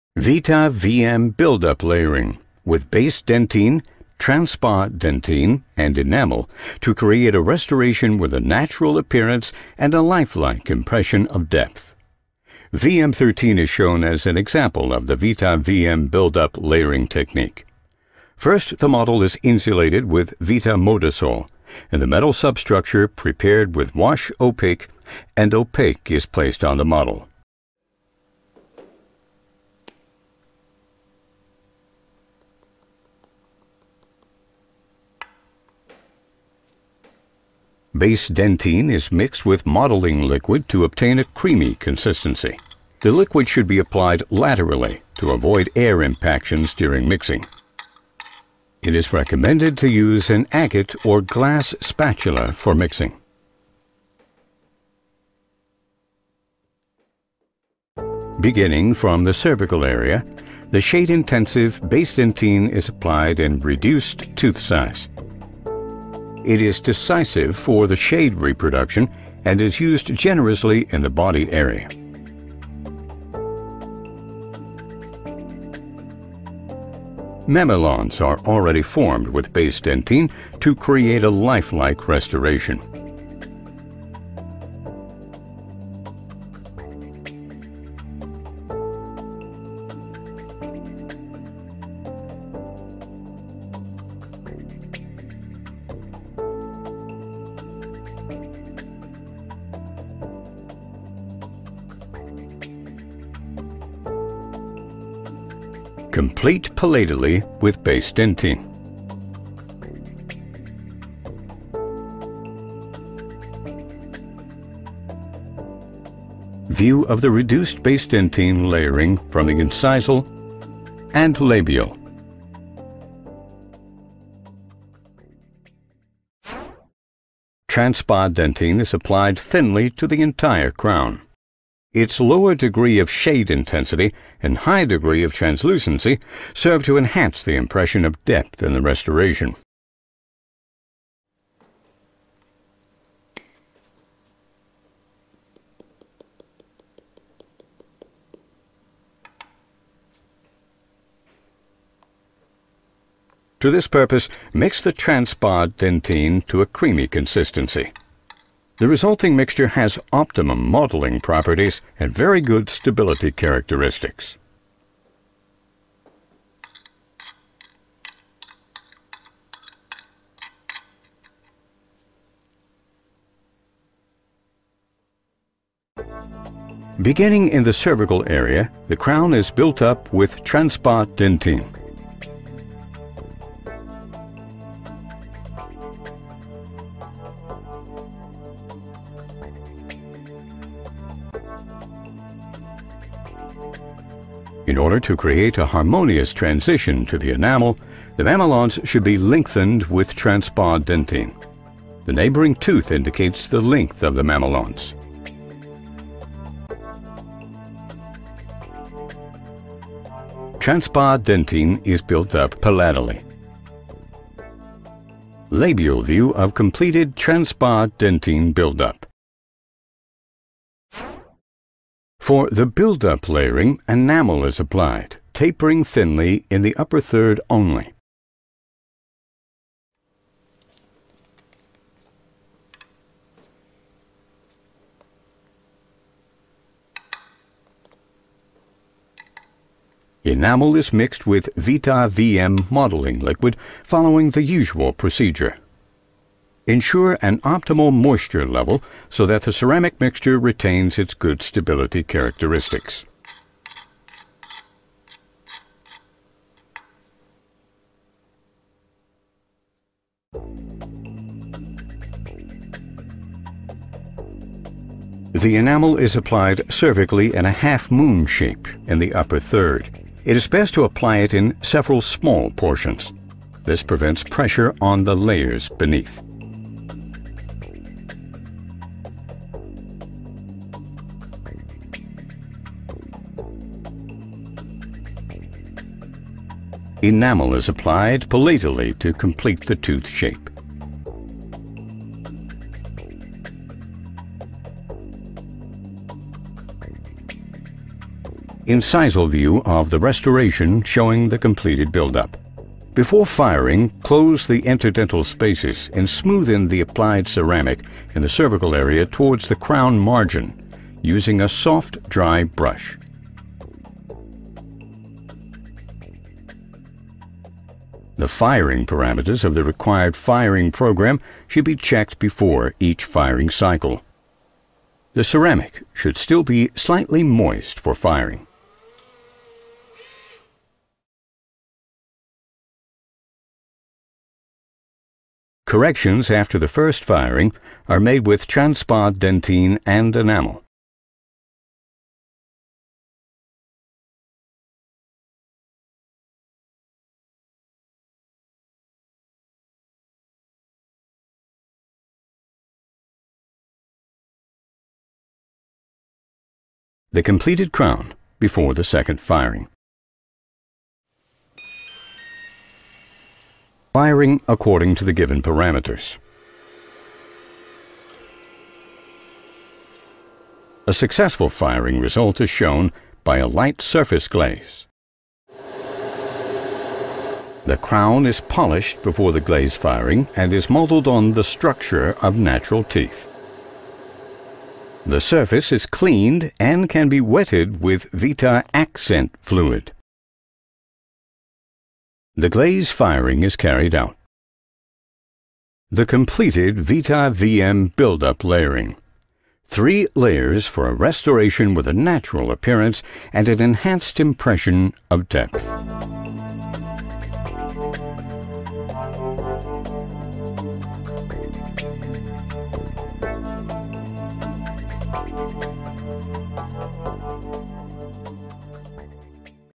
VITA VM Buildup Layering Technique
This brief video demonstrates the VITA VM 3-powder "Buildup Layering" method that can be used to produce highly esthetic restorations with ease. This procedure uses Base Dentine, Transpa Dentine and Enamel porcelain.

Take advantage of our Black Friday sale this week! Buy 3 bottles of VITA VM, get one free!